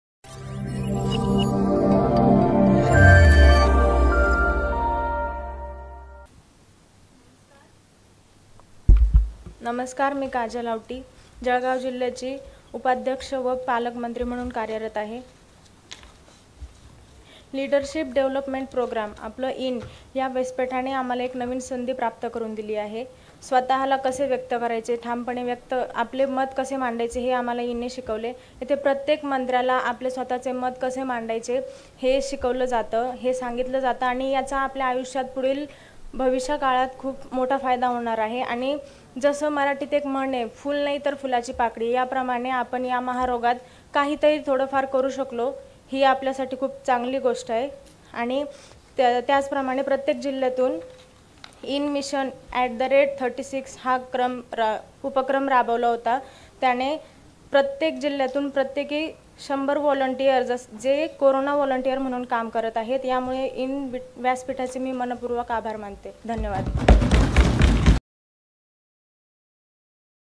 Kajal Aauti’s speech at the YIN Adhiveshan 2020-21 | YIN Ministry |YCMOU | Nashik| पालकमंत्री, जळगाव
काजल औटी – पालकमंत्री, जळगाव यांचे यशवंतराव चव्हाण महाराष्ट्र मुक्त विद्यापीठ, नाशिक येथे पार पडलेल्या यिन अधिवेशन २०२०-२१ मधील भाषण | दिवस पहिला |

Speech by Kajal Aauti at YIN Convention 2020-21 held at Yashwantrao Chavan Maharashtra Open University, Nashik